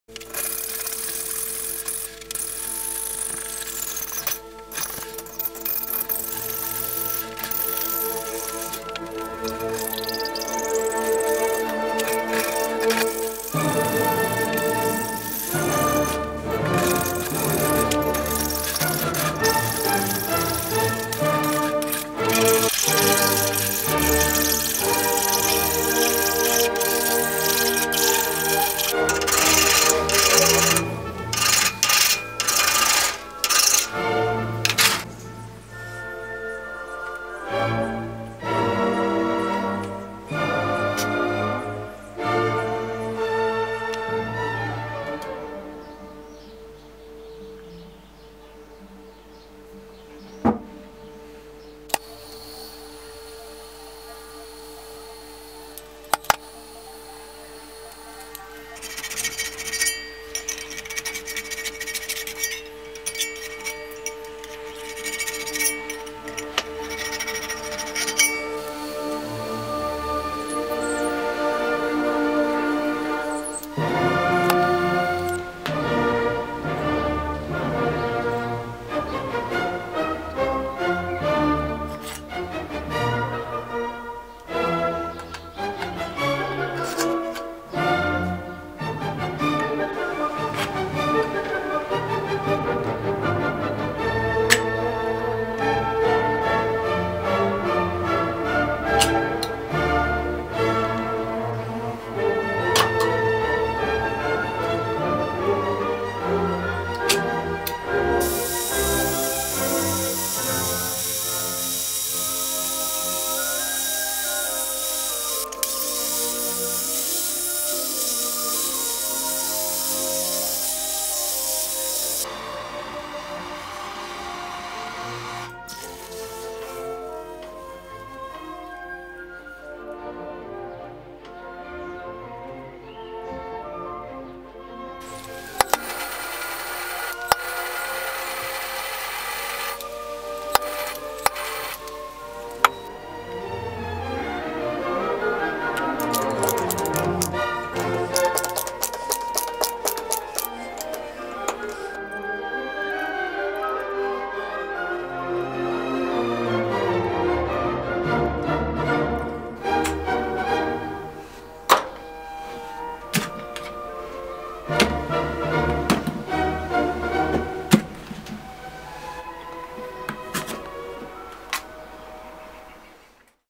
Making an Automatic Hidden Wallet Knife
I make a hidden wallet knife that can be extended at the flick of a switch.
The mechanism allows the blade to be retracted by being pushed against something.